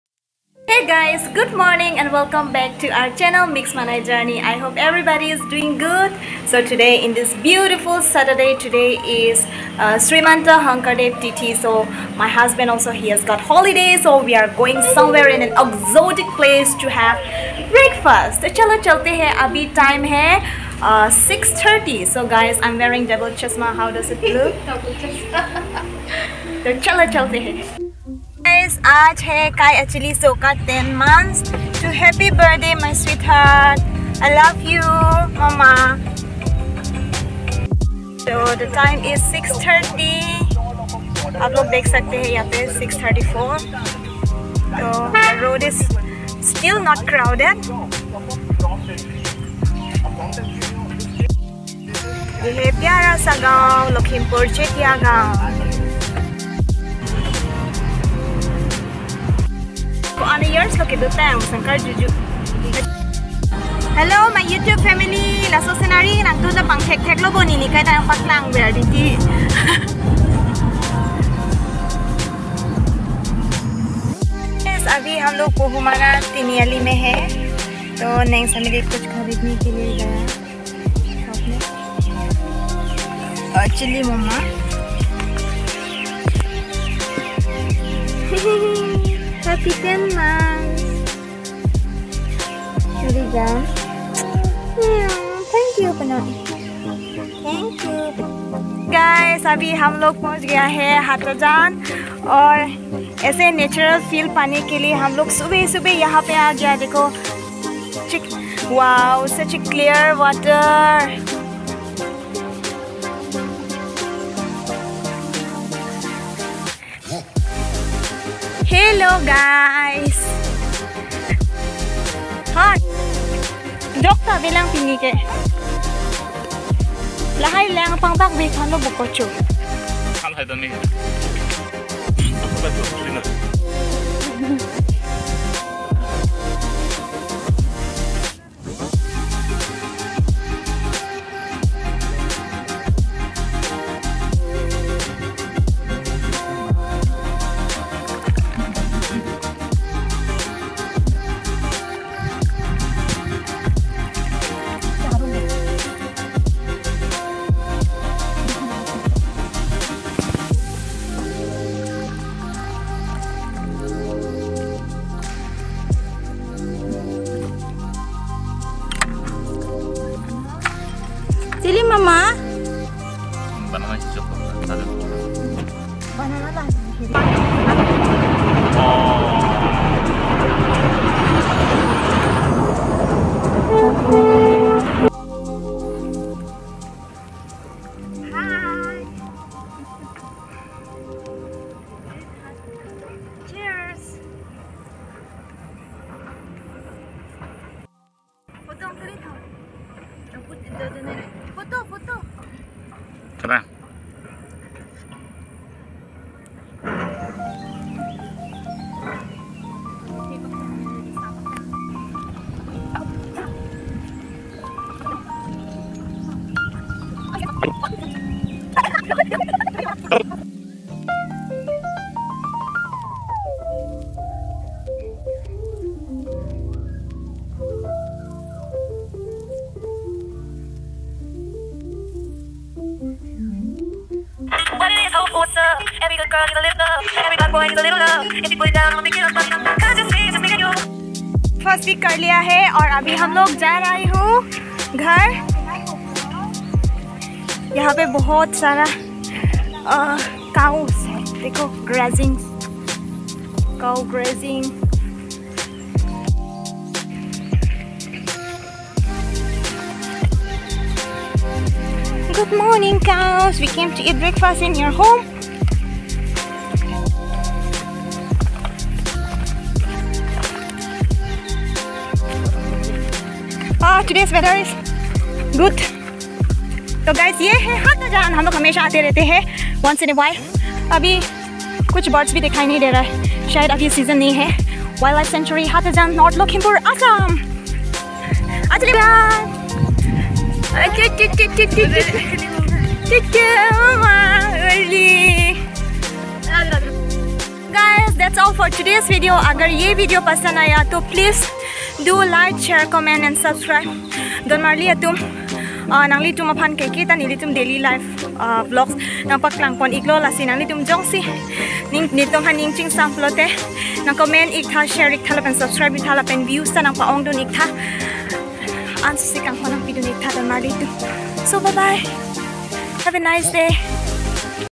Exotic Breakfast 🥞 at Satjan, North Lakhimpur 😊@miksmanai
On the 10th months of our baby we went on the bank of Ronganodi river ,beside Satajan bird scantuary North Lakhimpur,we had  breakfast  there.